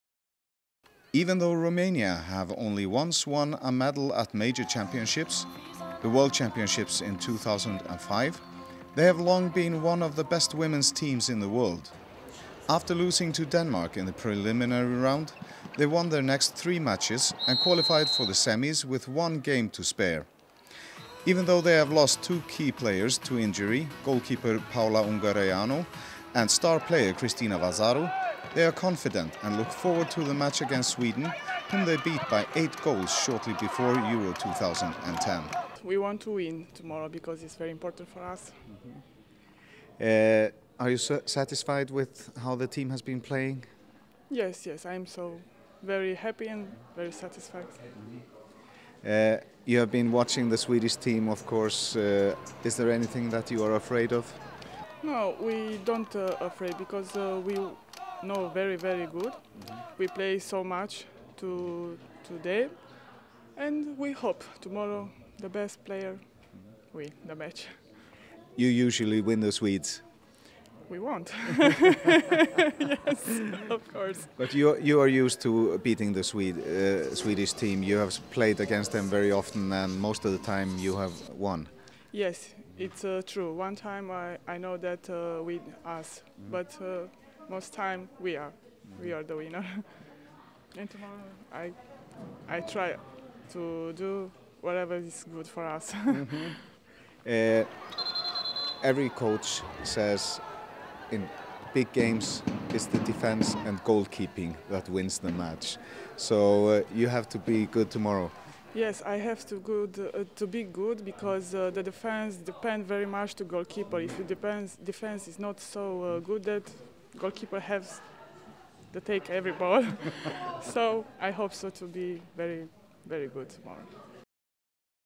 Romania confident before showdown with Sweden
The Romanian team are confident before their semi-final match against Sweden at EHF EURO 2010 in spite of losing two key players to injury